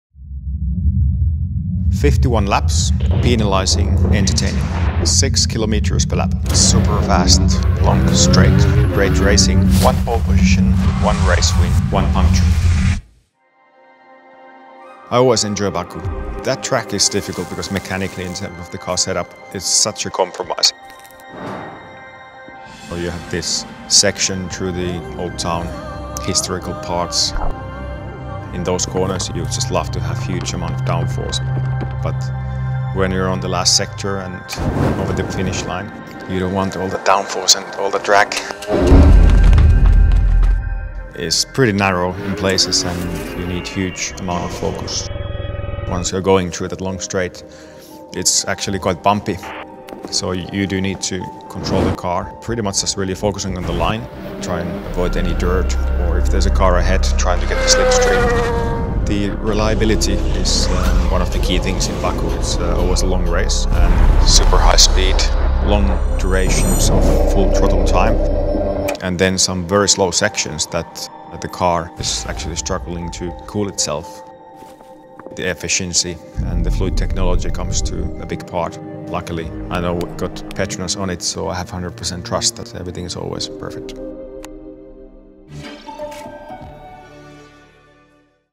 PETRONAS RACE PREVIEW - GP DO AZERBAIJÃO
Um circuito com uma reta de altíssima velocidade somado a trechos lentos e estreitos: conheça a pista do próximo desafio, em Baku, no Azerbaijão, e confira a importância de PETRONASSyntium para a equipe Mercedes-AMG Petronas Formula One Team